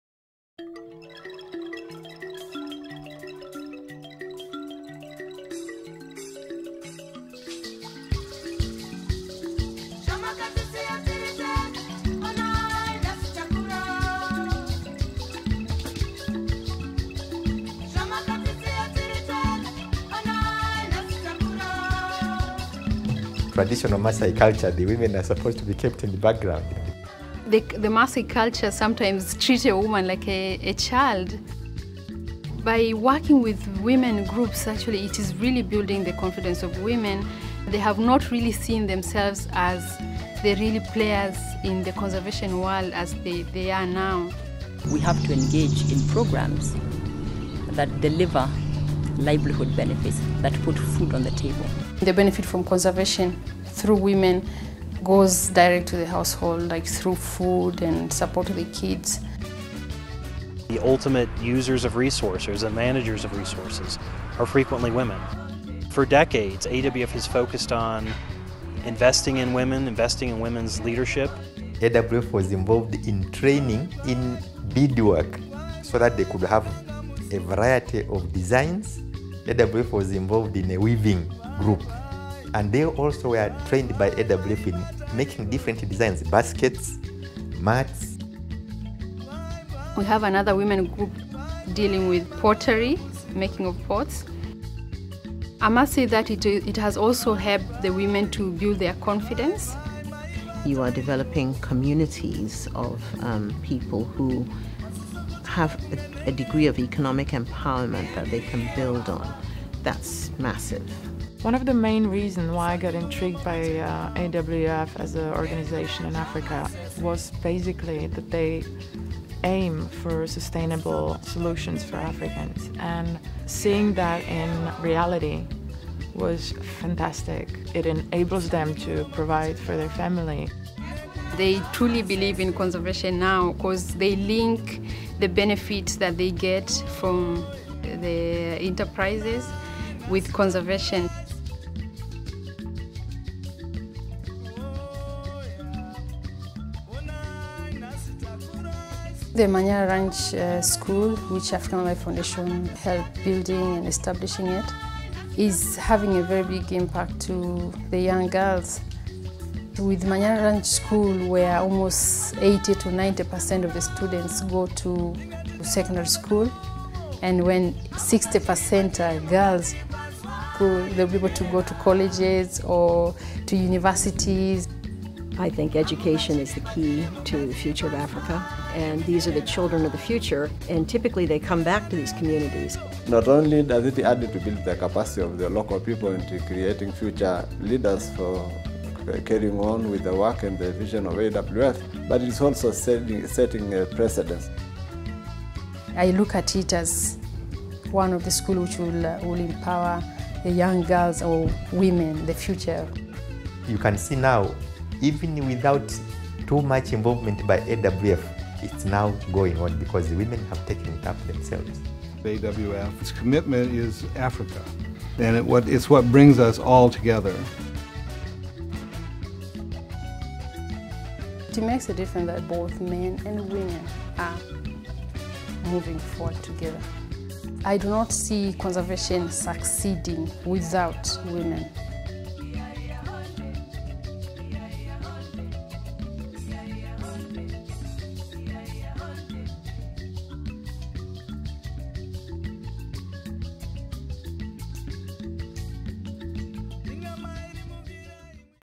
Women's Enterprise in Africa: Weaving a Brighter Future
Near Tarangire National Park, Tanzania sits a banda, or business stall, where the hands of the Mwada women weave not only baskets and mats but also a future in which they and their daughters will play a greater and more meaningful role within their homes, their communities, and their country. AWF, with support from USAID, constructed this banda to serve as a store where they can sell handcrafts to tourists visiting the nearby national park. The Mwada venture is only one in a string of successful women-run enterprises that were established with the help of AWF that link conservation to economic benefits.